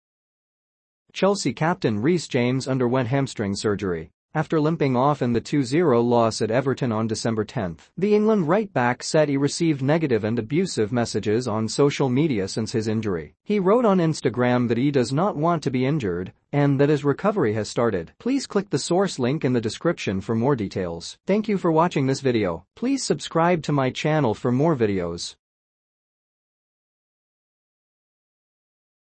Reece James slams online abuse after hamstring surgery
Chelsea captain Reece James underwent hamstring surgery after limping off in the 2-0 loss at Everton on Dec. 10. The England right back said he received negative and abusive messages on social media since his injury. He wrote on Instagram that he does not want to be injured and that his recovery has started.